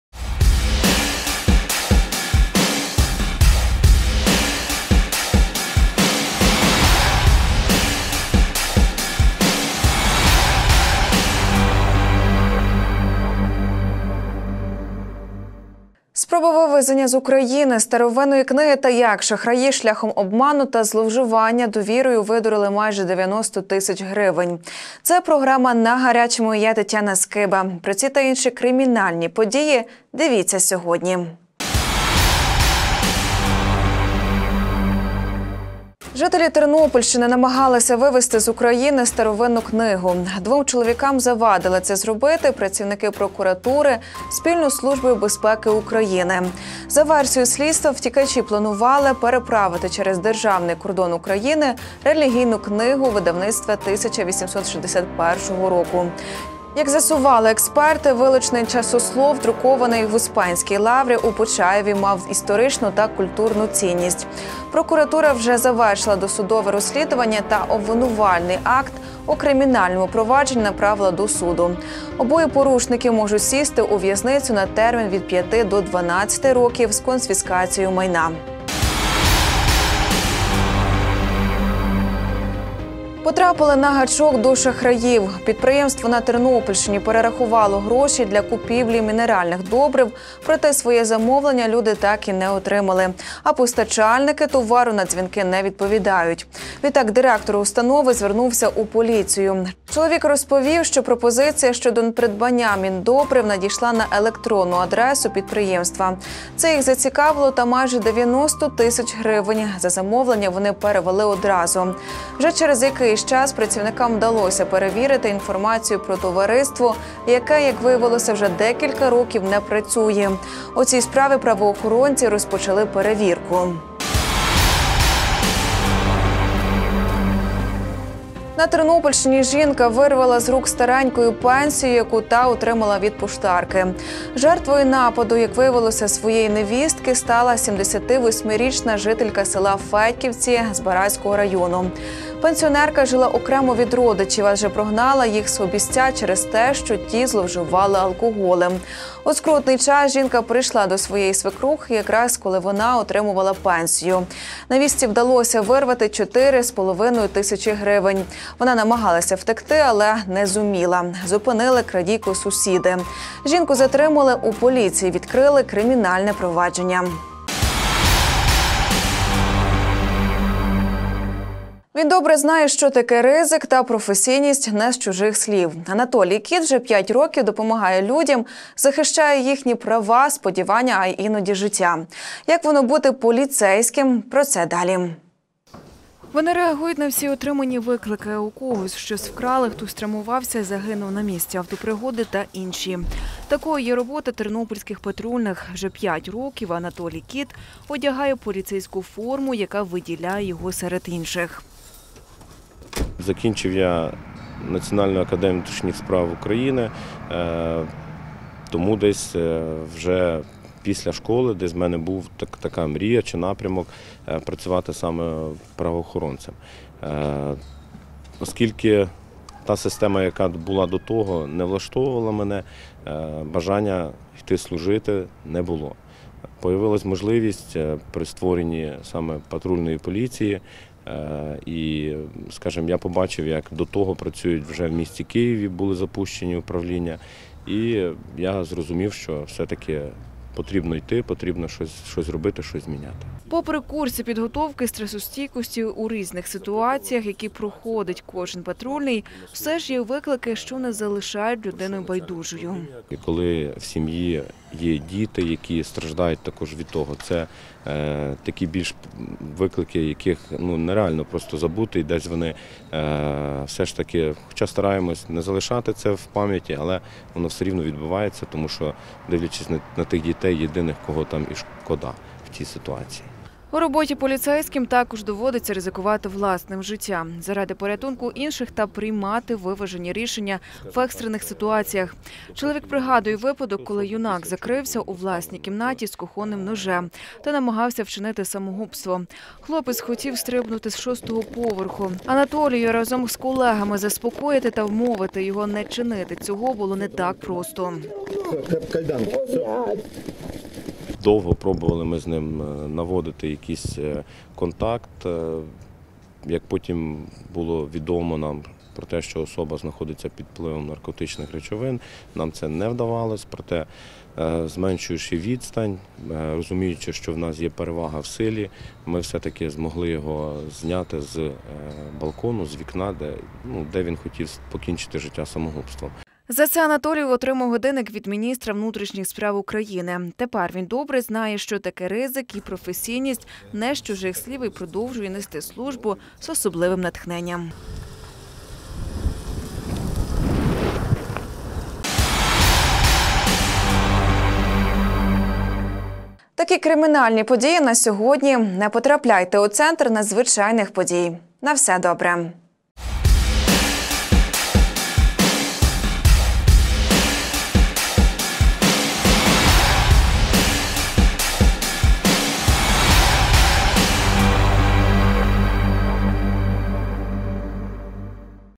Надзвичайні події "На гарячому" (17.03. 2020)
00:36 - Жителі Тернопільщини намагалися вивести з України старовинну книгу.
01:23 - Підприємство на Тернопільщині перерахувало гроші для купівлі мінеральних добрив, проте своє замовлення люди так і не отримали.
02:08 - На Тернопільщині жінка вирвала з рук старенької пенсію, яку та отримала від поштарки.
02:52 - Анатолій Кіт вже п'ять років допомагає людям, захищає їхні права, сподівання, а іноді й життя. Як воно бути поліцейським - про це далі.